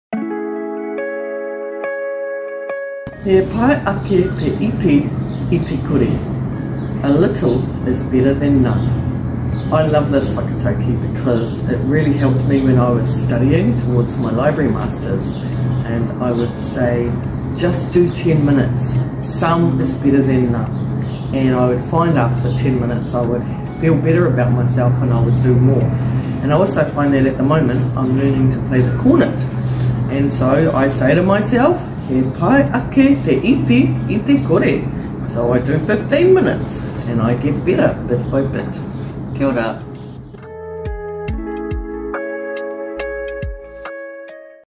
Whakataukī: He pai ake te iti i te kore
Nā Anne te whakataukī i tohaina ki koe i tēnei Rāhina. University study can often feel really overwhelming, so this whakataukī encourages us to do a little. With a lot of little steps, we can complete our tasks, and move closer to our goals! Karawhiua!"